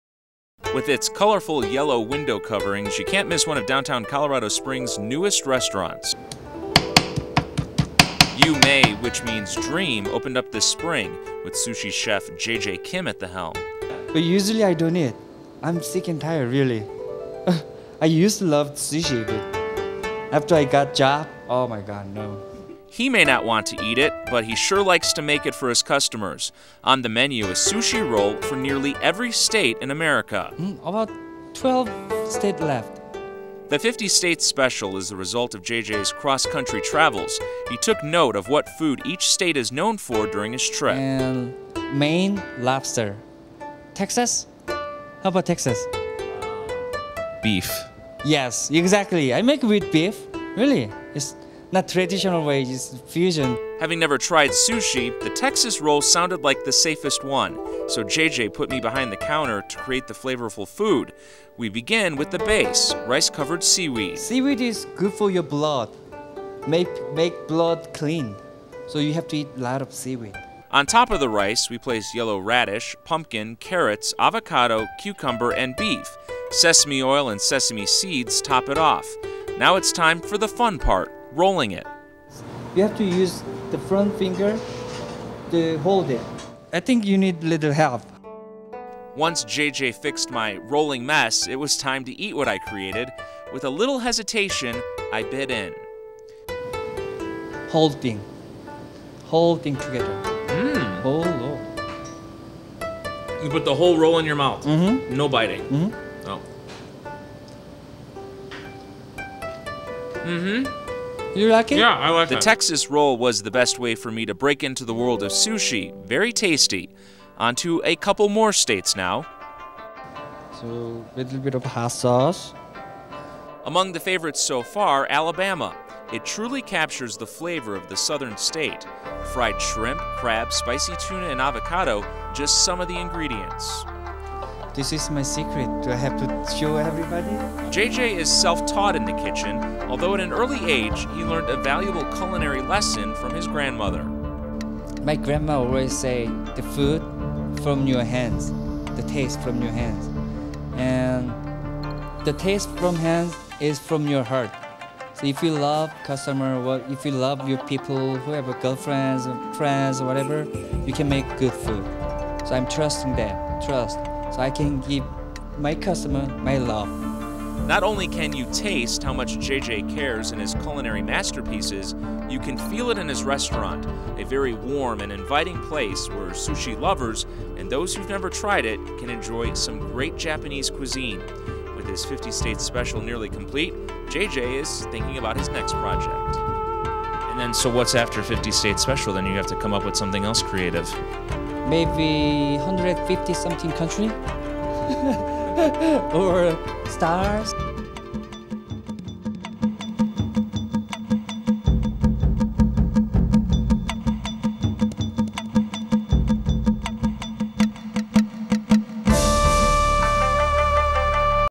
Eugene Armstead Shoots and Edits Unique Eats: Yoo Mae
This "Unique Eats" package spotlights the Japanees restaurant and sushi bar Yoo Mae in Colorado Springs, Colorado.  In this video you will see Mike Brookbank interviewing the store's owner JJ Kim.